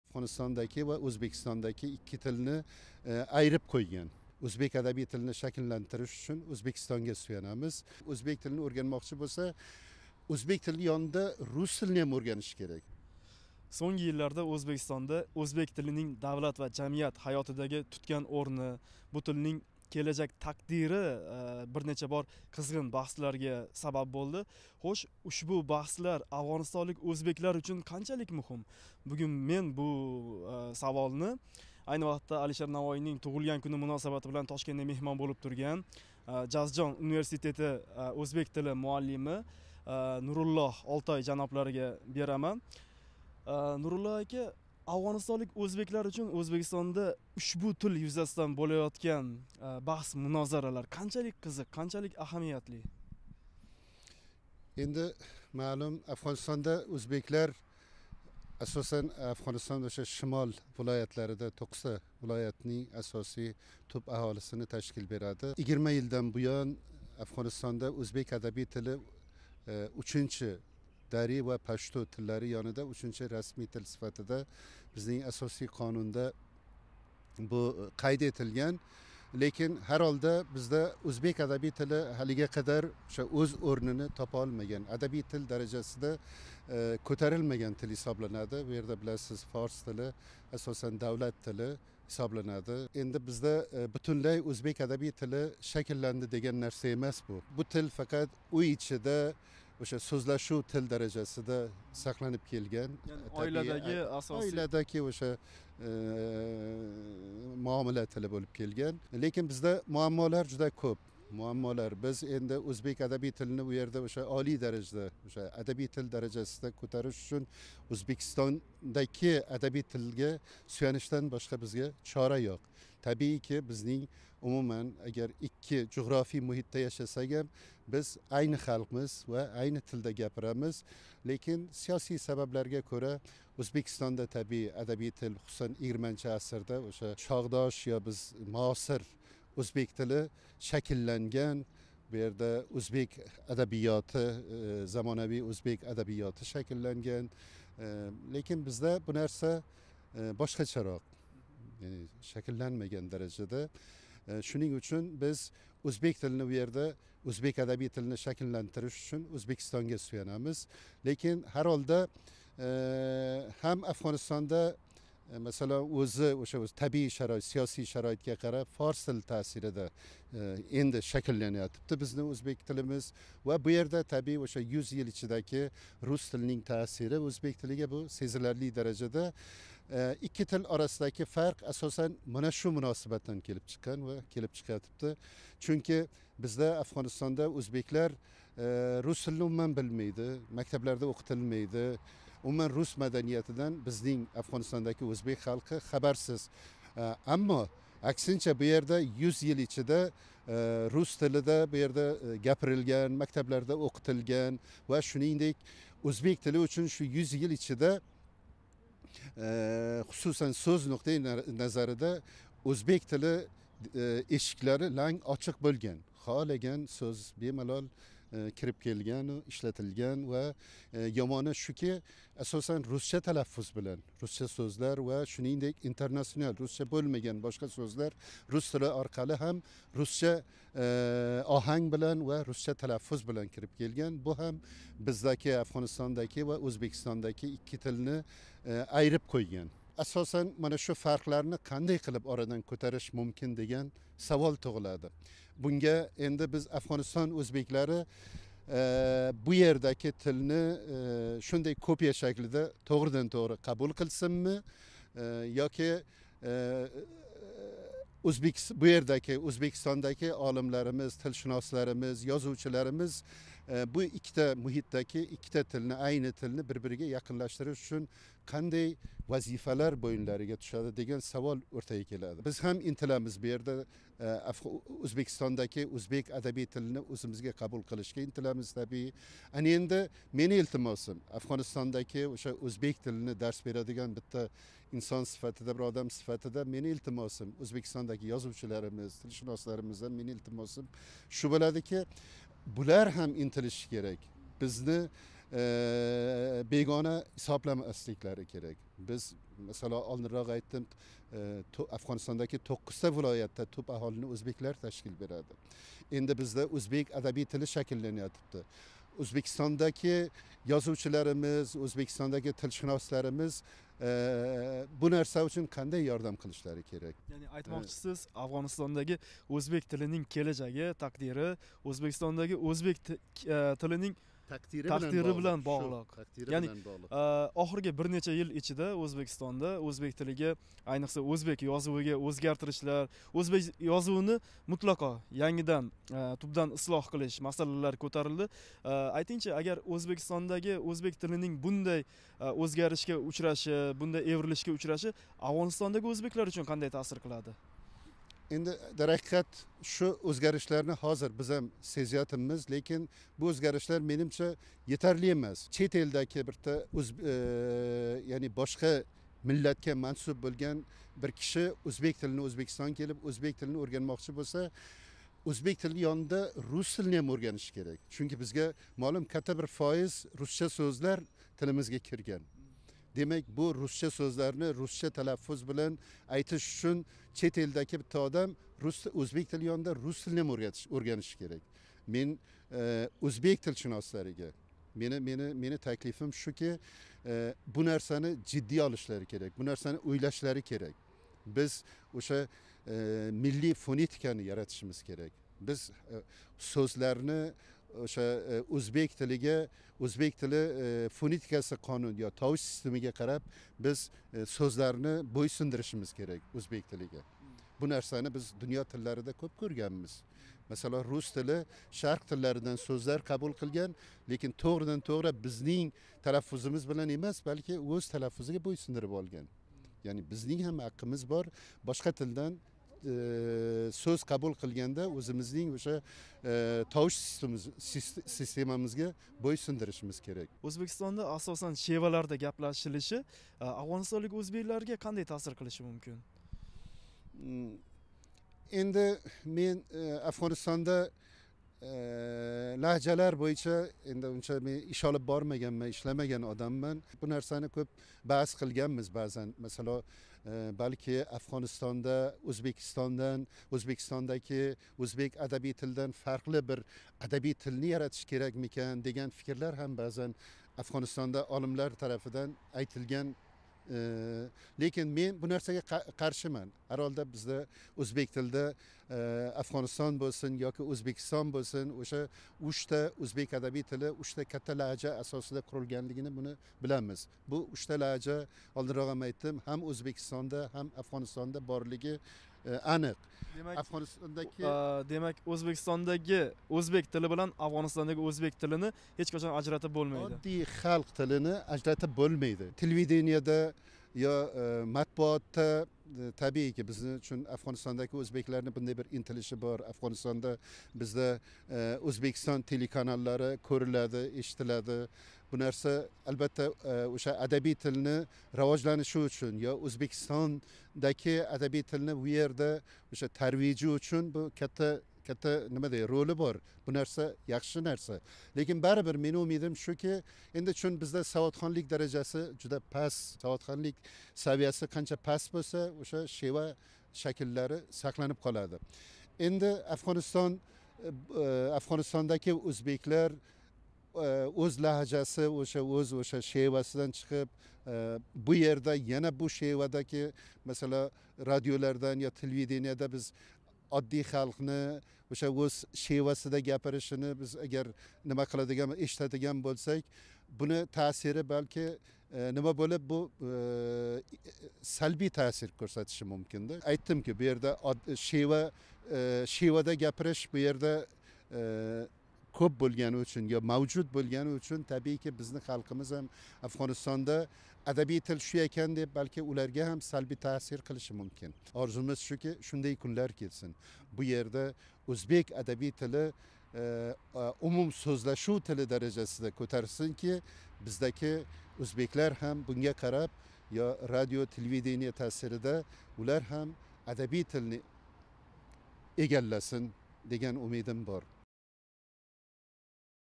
Ўзбекистон: Афғонистондаги ўзбек тили тақдири Ўзбекистонга боғлиқми? O'zbekiston, Uzbekistan - BBC
Ўзбекистон: Афғонистондаги ўзбек тили тақдири Ўзбекистонга боғлиқми?
9 февраль куни Тошкентда Алишер Навоий таваллудининг 579 йиллигига бағишланган тадбирлар бўлиб ўтди. Бу тадбирларда Туркия, Озарбайжон ва Афғонистондан меҳмонлар иштирок этдилар. Афғонистонлик меҳмонлар қаторида бўлган ўзбек тили мутахассиси Нуруллоҳ Олтой Би-би-сига ушбу тилнинг Афғонистондаги тақдири борасида фикрларини баён этди.
Нуруллоҳ Олтой афғонистонлик ўзбеклар орасида ўзбек тилининг етук мутахассиси ўлароқ танилган.
У Афғонистон шимолидаги Жузжон Университетида ўзбек тилидан дарс беради.
Жаноб Олтойнинг фикрича, Афғонистон ва Ўзбекистон ўзбеклари «аслида бир миллат», аммо улар сўзлашадиган ўзбек тилидаги юзага келган фарқлар «бу миллатни бир-биридан бегоналаштириб юбормоқда».